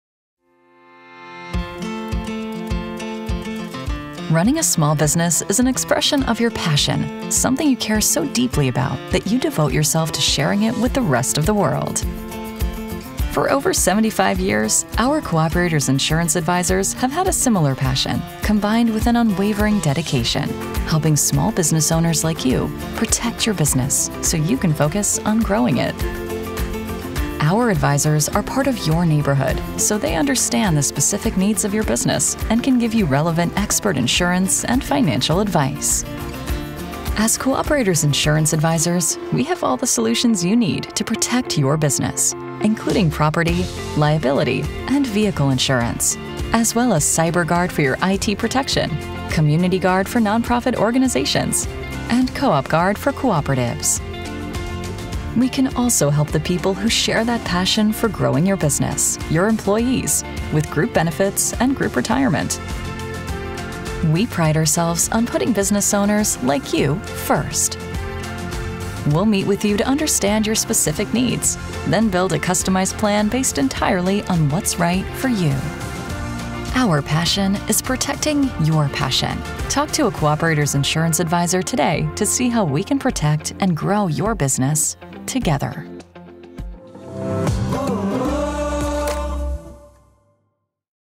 Protecting your small business
We pride ourselves on putting business owners, like you, first. We’ll meet with you to understand your specific needs, then build a customized plan based entirely on what’s right for you. Our passion is protecting your passion.

Talk to a Co-operators insurance advisor today to see how we can protect and grow your business. Together.